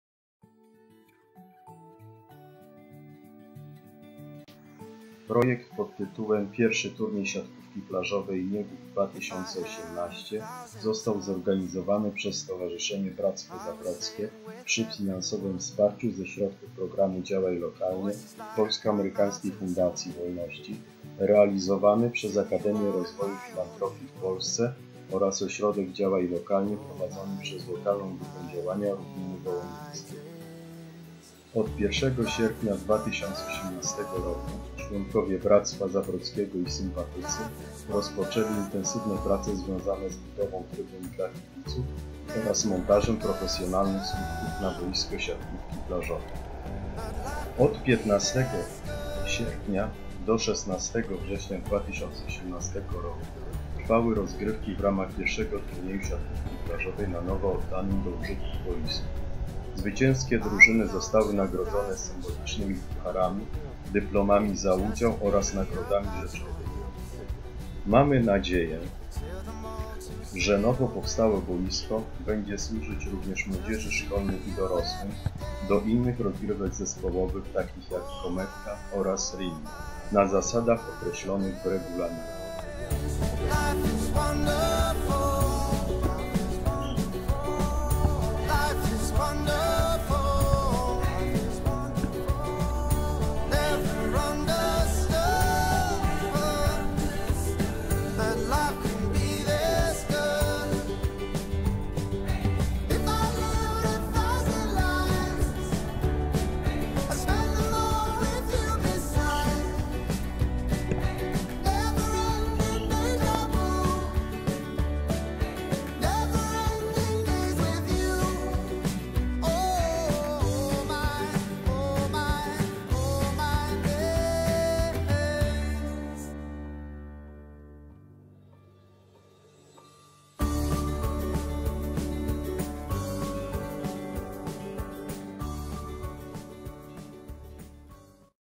Turniej Siatkówki Plażowej
Prezentuje prace wykonane wspólnie przez zaangażowane osoby, na boisku (montaż słupków) oraz budowę trybun dla kibiców. Punktem kulminacyjnym było przeprowadzenie Turnieju Siatkówki Plażowej. Projekt został dofinansowany ze środków "Działaj Lokalnie X" P-AFW.